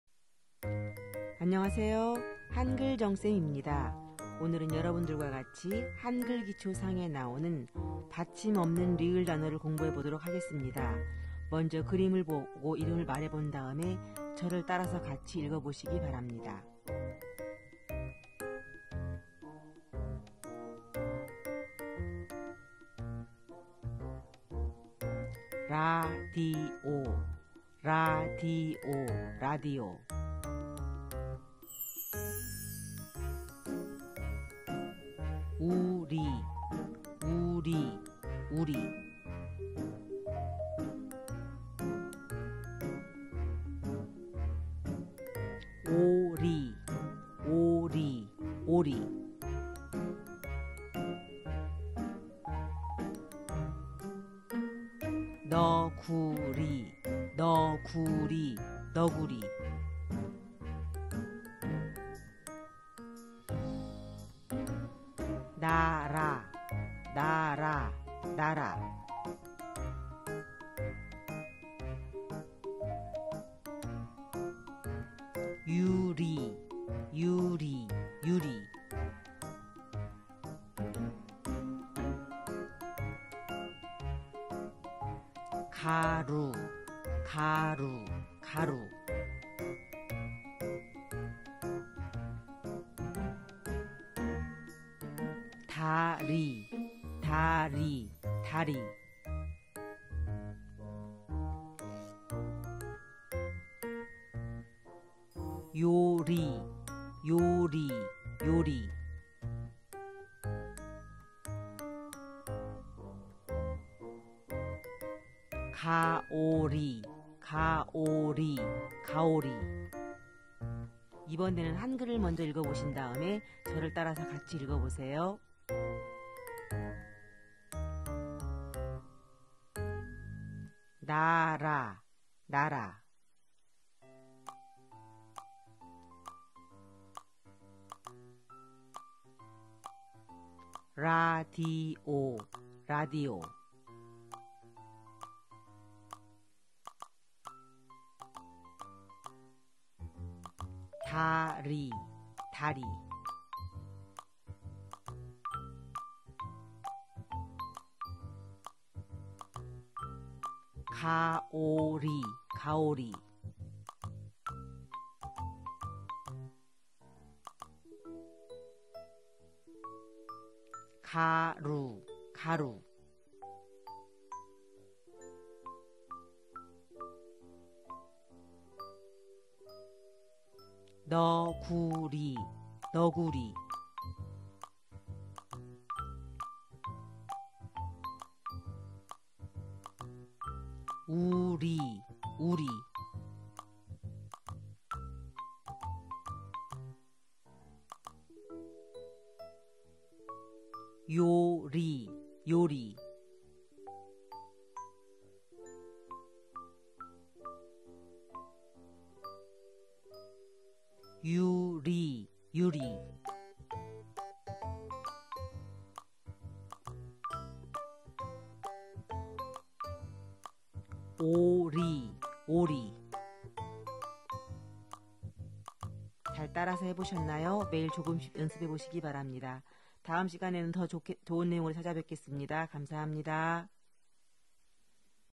[한국어단어공부] ㄹ 받침 없는 단어 Korean Vocabulary
언어를 배울 때 가장 중요한 것 중의 하나가 단어를 많이 아는 것입니다. 그러나  미국에서 자라는 아이들은 가정보다 학교에서 보내는 시간이 많기 때문에 우리말에 노출될 기회가 적습니다.  단어를 많이 접하기 어려운 환경이다 보니  한글의 기본원리를 깨치고 나서도 진도가 더 앞으로 나가기 어렵습니다.  집에서 부모님의 도움을 받기가 어려운 학습자들을 위해서 각 단원별로 교과서에서 다루는 중요 단어를 중심으로 단어 공부 동영상을 만들었습니다.

단어는 재외동포 교육진흥재단에서 발행한 한글 기초(상)을 중심으로 선정한 단어들입니다.
매일 5분씩만 투자해서 단어를 공부하도록 아이들을 지도해 주세요.